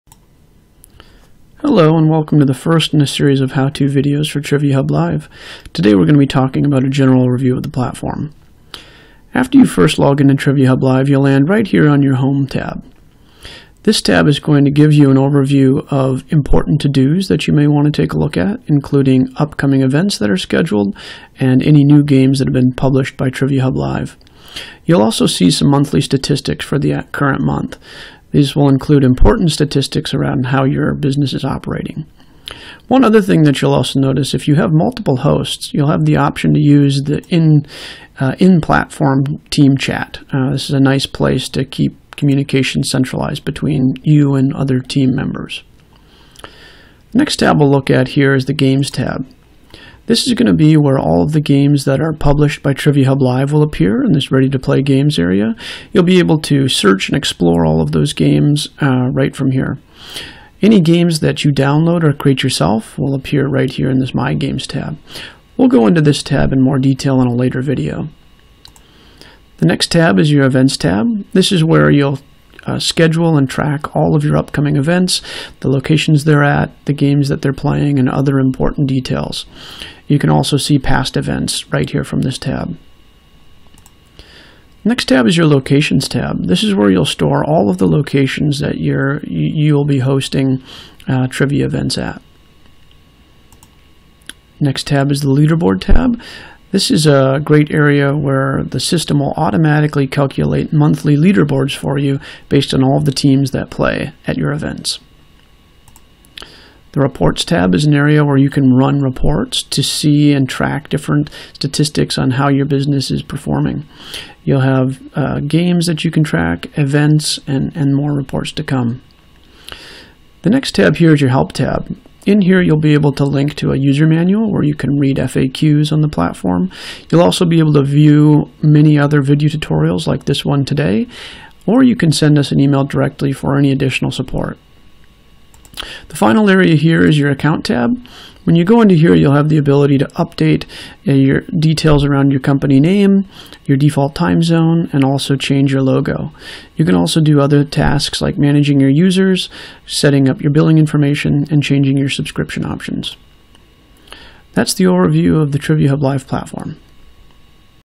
How To: TriviaHub Live Introduction
TriviaHub Live is a software platform for hosting the perfect trivia night. Watch this video to get an introduction to all of the current features of the platform.